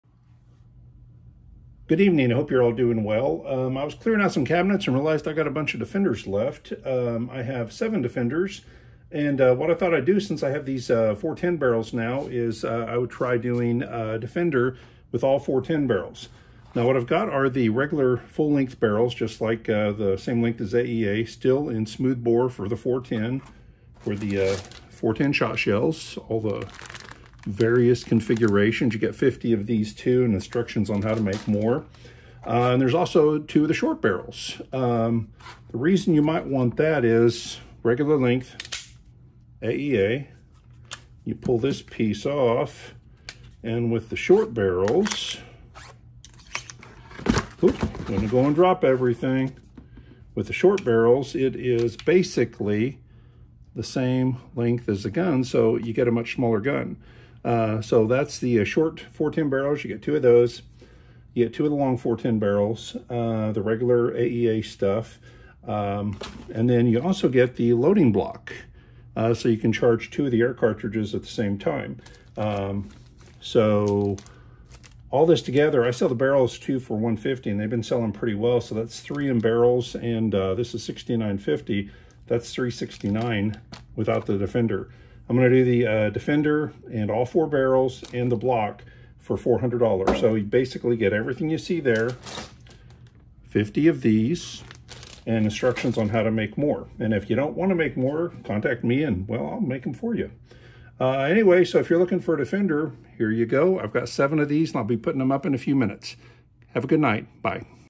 Custom AEA Defender 4 shotgun barrel package by Texoma Precision Pellet VID95
We were doing some house cleaning and found seven Defenders and decided to package them with our 410 barrels.